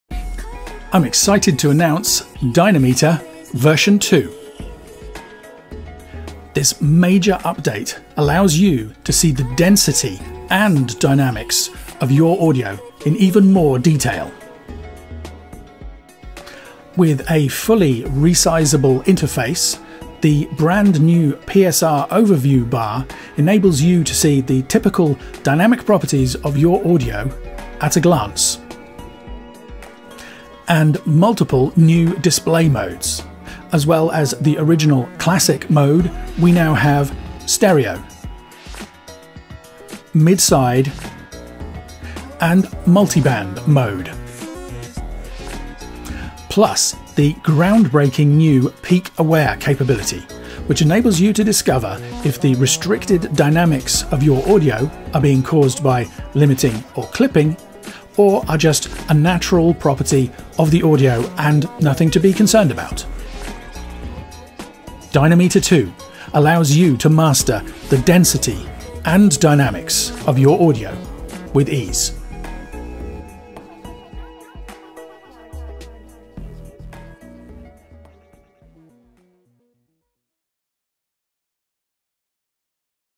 What's New in Dynameter 2?
Mastering Engineer Ian Shepherd introduces our Dynameter 2 plugin, which helps you discover and tune the dynamics and density of your audio.

New features in version 2 include:
* New display modes: Stereo, Mid / Side and Multiband
* Peak Aware display: See if your dynamics are limited due to clipping or if your audio is naturally less dynamic
* PSR Overview histogram: A quick breakdown of your audio's dynamics
* Clip indicators: Now visible in the PSR History graph
* Fully resizable UI